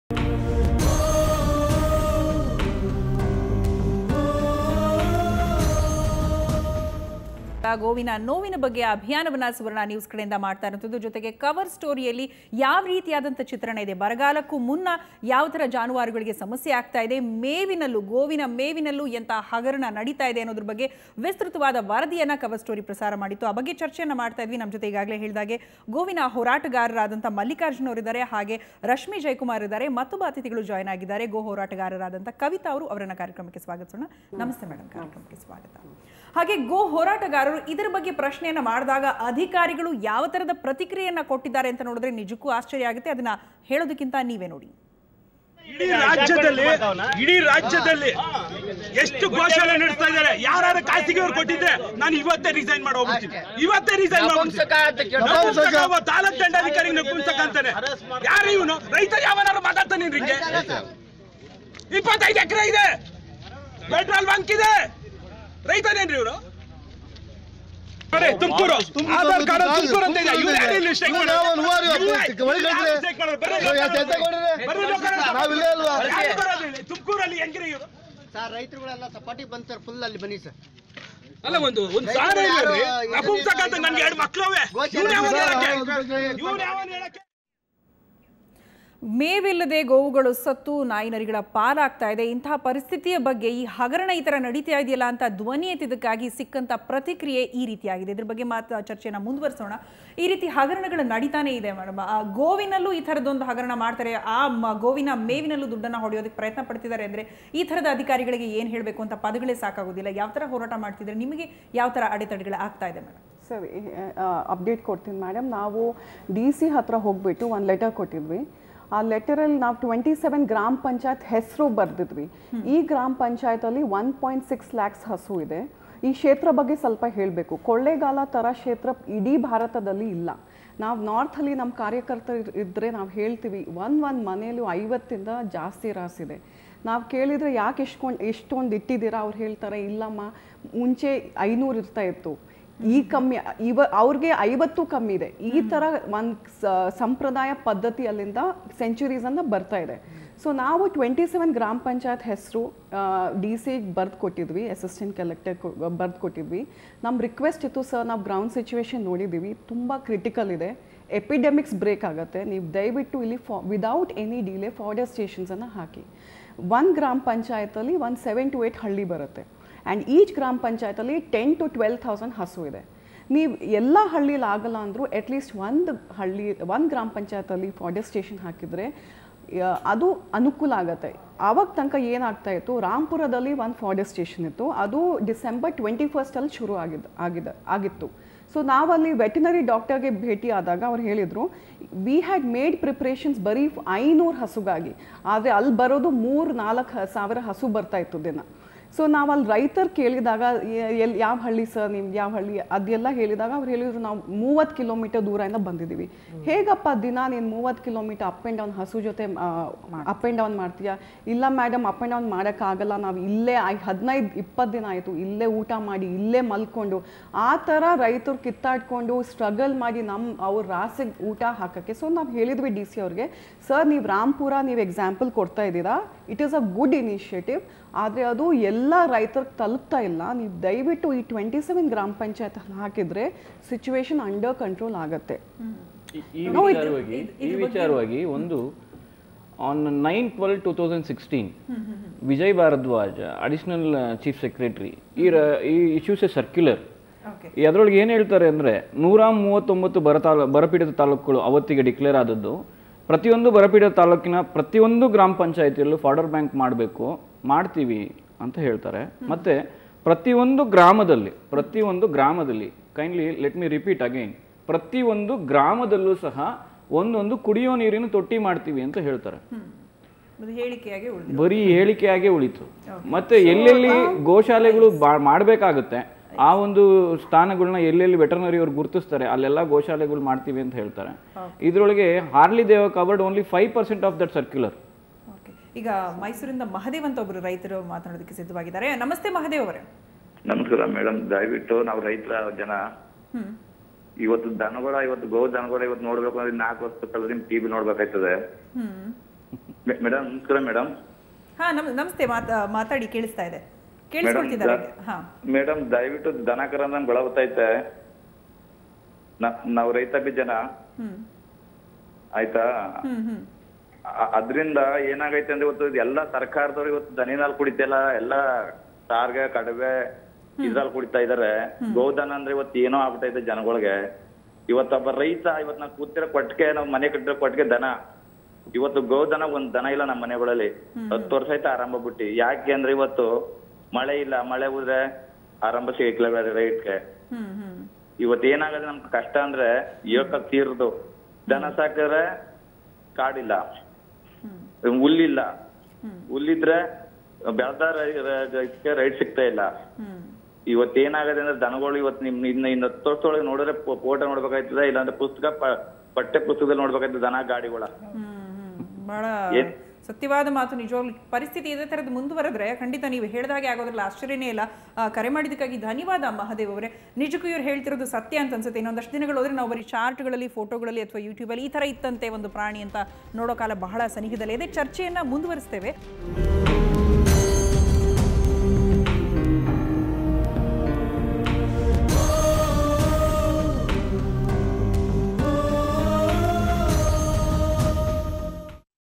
Goovina Novu Special Discussion About 10th March 2017 Cover Story Part 3
ಲಕ್ಷ ಲಕ್ಷ ಗೋವುಗಳನ್ನು ಕೈಯ್ಯಾರೆ ಕೊಲಲುತ್ತಿರುವವರು ಯಾರು ಗೊತ್ತಾ? ಕವರ್ ಸ್ಟೋರಿ ಯಿಂದ ಬಯಲಾಯ್ತು ಕರಾಳ ಸತ್ಯ.. ಸರ್ಕಾರದ ಮೇವು ಹಗರಣಕ್ಕೆ ಬಲಿಯಾದವು ಪಾಪದ ರಾಸುಗಳು